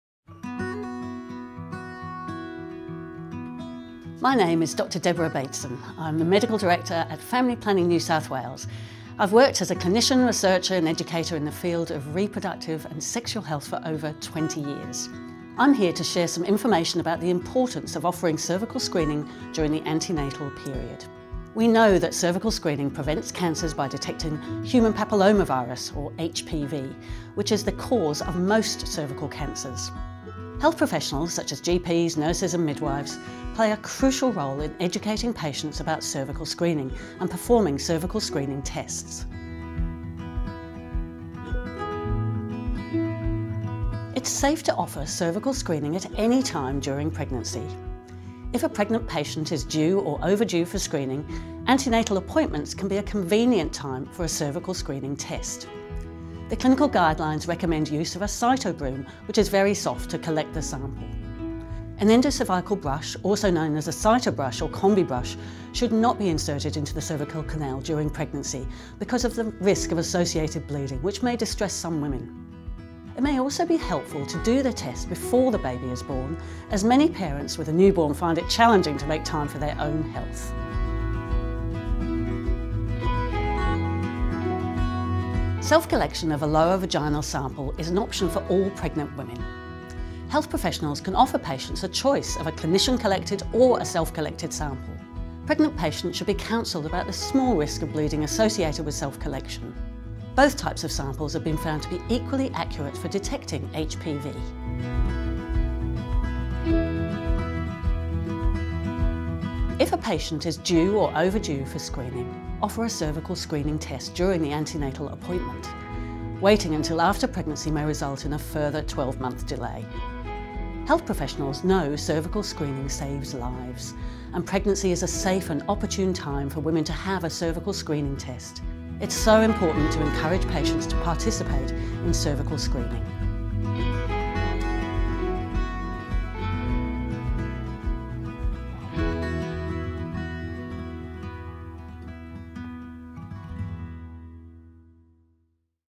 Cervical screening during pregnancy with Dr Deborah Bateson
The National Cervical Screening Program clinical guidelines recommend cervical screening at any time during pregnancy provided that the correct sampling equipment is used. This includes both options - the clinician-collected sample or a self-collected sample. Health professionals, such as GPs, nurses and midwives, play a crucial role in educating patients about cervical screening and performing Cervical Screening Tests.